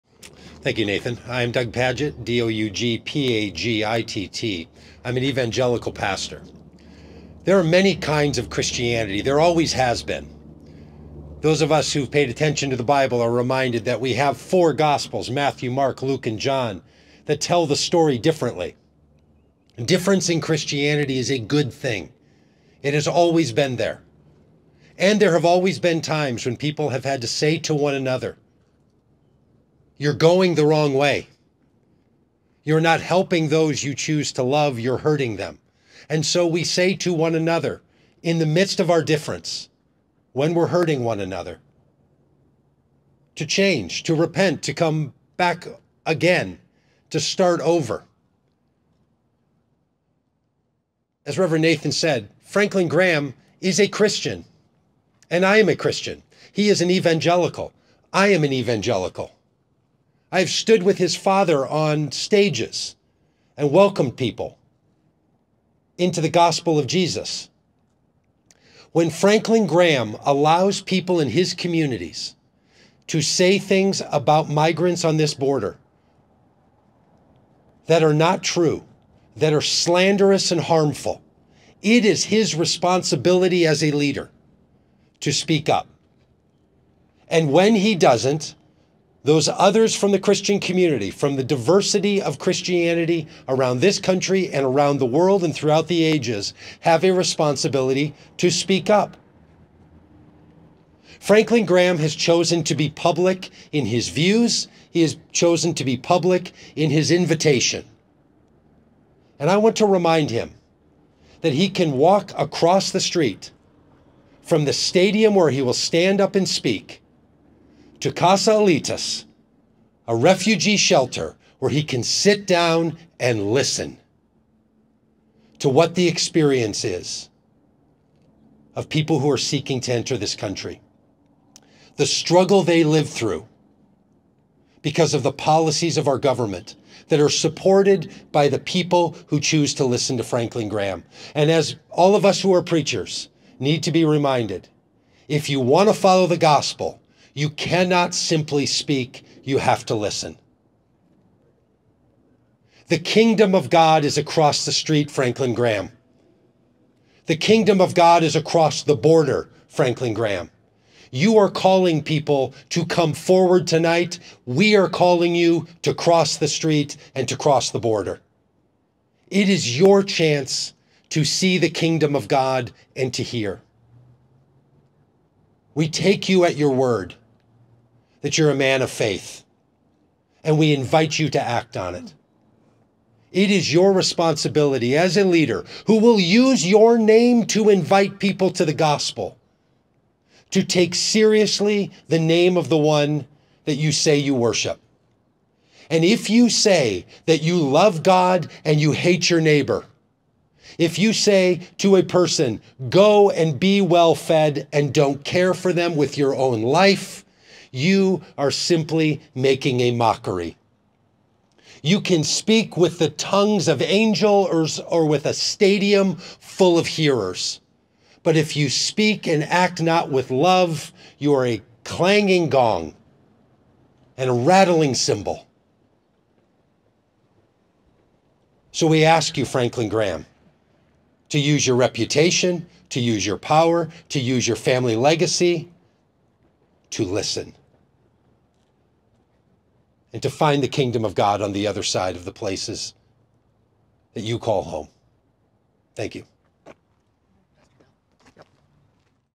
PRESS EVENT: Doug Pagitt speaks out against Franklin Graham Tour
From the Faithful Response to Franklin Graham press event in Tucson, Arizona.

Doug Pagitt, an evangelical pastor and the executive director of Vote Common Good, speaks out against Franklin Graham’s border tour.

The Franklin Graham tour is a thinly veiled effort to demean migrants and Asylum seekers and to recruit attendees into a version of American Christendom that coddles Christian Nationalists and political extremism.